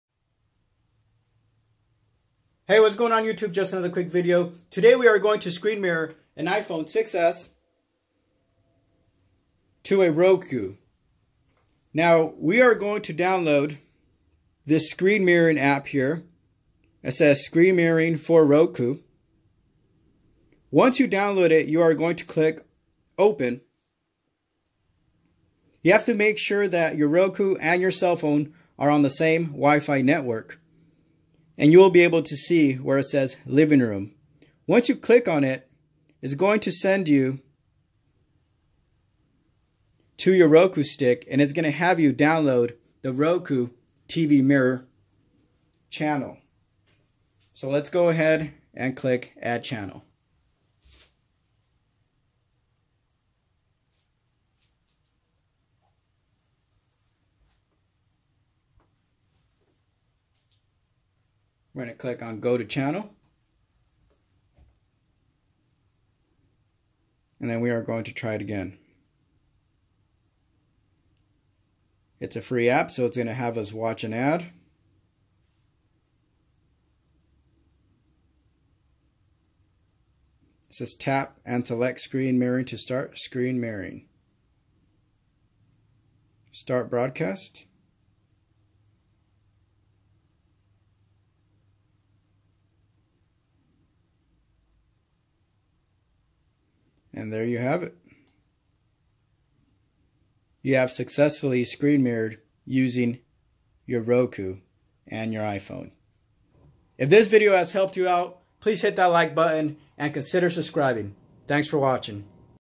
SCREEN MIRROR I PHONE TO ROKU
APPLE TV
CURVED 32 IN SAMSUNG MONITOR
50 Inch Sharp Roku TV
TCL ROKU TV
ROKU EXPRESS
AMAZON FIRE STICK
(Hard wire) USB C TO HDMI ADAPTER

There are many apps you can download to screen mirror your I Phone to the Roku. This App  I downloaded worked for me and was simple to install. A better way to screen mirror your I Phone is to do it in combination with the Apple TV.